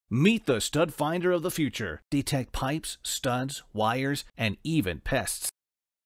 Walabot Diy Stud Finder
Walabot DIY - In-Wall scanner - See Studs, Pipes, Wires.
The tool is a cutting-edge device which is paired with an Android phone to produce a visual image of what is inside concrete and drywall up to 4 inches (10 cm) deep.